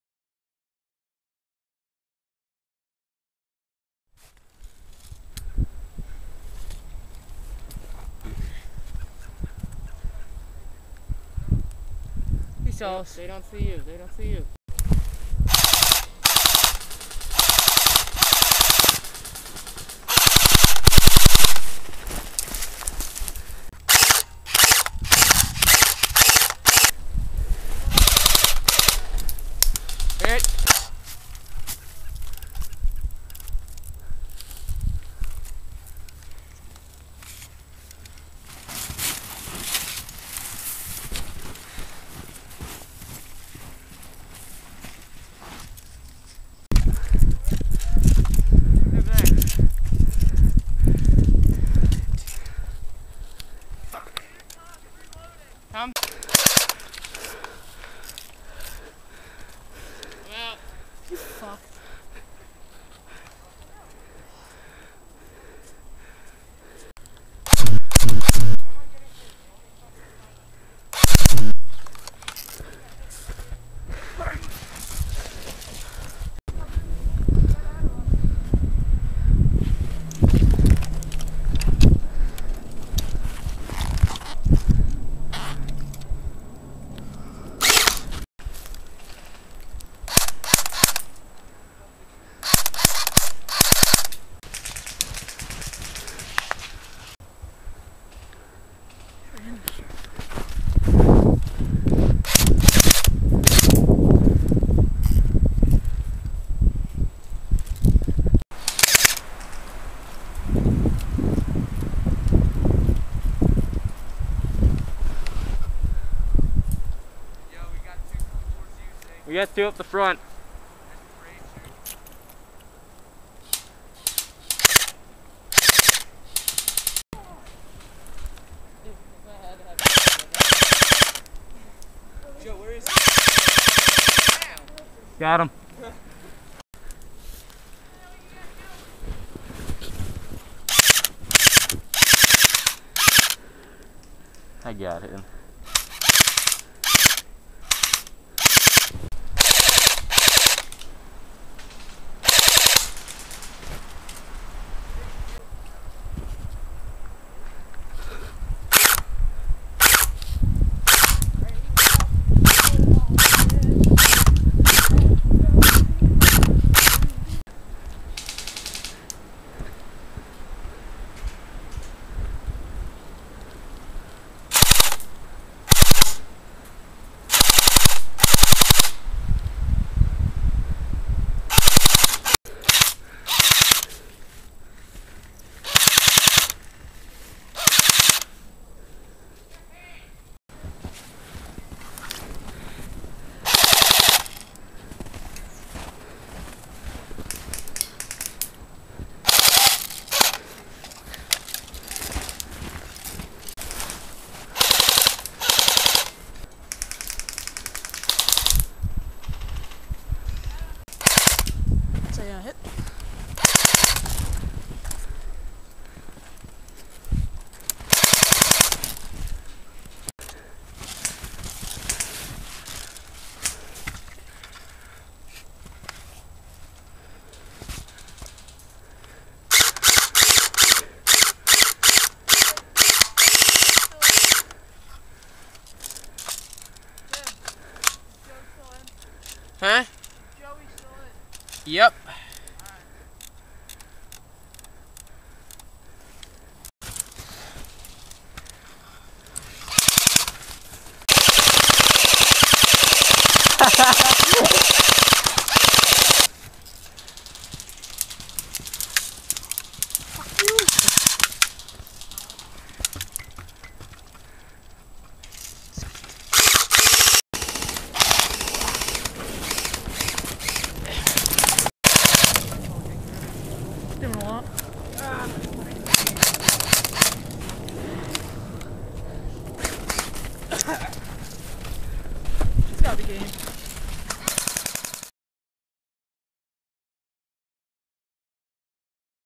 Airsoft Wall Defence-M4, JG-SP3, ICS L86
Some airsoft action filmed in some thick snow.  These are clips from a wall defence game at "The Farm".  Enjoy this video as you watch players with guns such as the amazing L86, go head to head in this defence game.